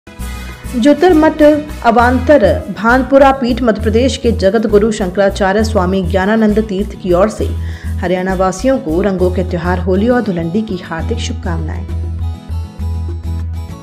स्वामी ज्ञानानंद तीर्थ की ओर से होली और धूलंडी की हार्दिक शुभकामनाएं
ज्योतिर्मठ अवान्तर भानपुरा पीठ मध्य प्रदेश के जगदगुरु शंकराचार्य स्वामी ज्ञानानंद तीर्थ की ओर से हरियाणावासियों को रंगों के त्यौहार होली और धूलंडी की हार्दिक शुभकामनाएं।